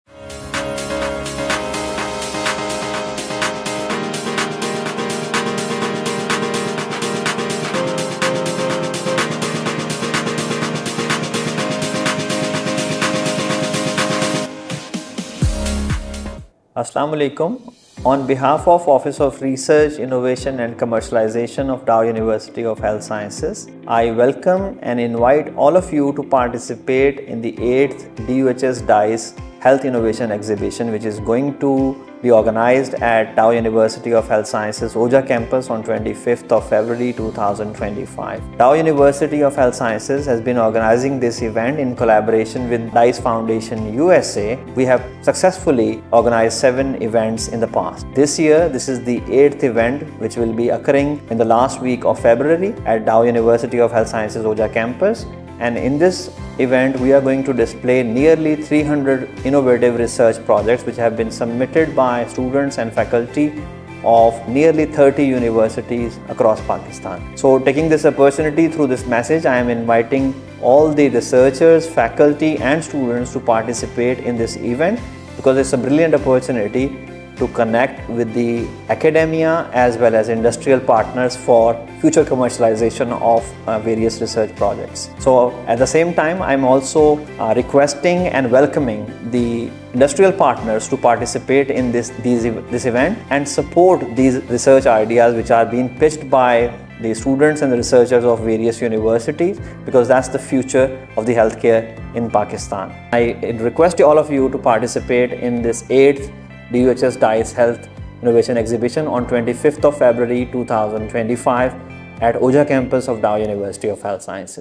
Invitation 8th All Pakistan DUHS-DICE Health Innovation Exhibition | 25 Feb 2025 | DUHS Ojha Campus
Dear Colleagues,
I am pleased to inform you that Dow University of Health Sciences in collaboration with Distinguished Innovations, Collaboration & Entrepreneurship - DICE Foundation is going to organize the 8th All Pakistan DUHS–DICE Health Innovation Exhibition on February 25, 2025 at DUHS Ojha Campus.
This distinguished and unique event will provide a platform to our youth and the entrepreneurs to share their innovative ideas and projects for the health industry as well as to the capitalists from Government and Private sector and the expatriates to interact jointly and share knowledge at par with the developed countries as far as possible.
By the grace of Allah, we have successfully hosted seven health innovation exhibitions in the year 2015, 2016, 2017,2018,  2019, 2021 and 2022 respectively, now we are heading towards the 8th event with the aim to make it more productive than the previous ones.
The main objectives of this exhibition are:
1.  To establish a strong linkage between Academia and Health providing institutions.
2.   Motivate professionally educated youth to generate innovative ideas and utilize currently available state of art technologies and software, for practically possible development of health-related products and services.
3.  To foster a culture of innovation and entrepreneurship in the country and make it a part of the Nation’s DNA.
4.    Globally creating a positive and favorable image of Pakistan’s health sector.
 Therefore, I would like to request your Institute/college to participate in this mega event through as many innovative ideas/projects as you can to highlight your esteemed institution in this revolutionary and distinguished experience. The last date for submission of the project/idea is December 30, 2024.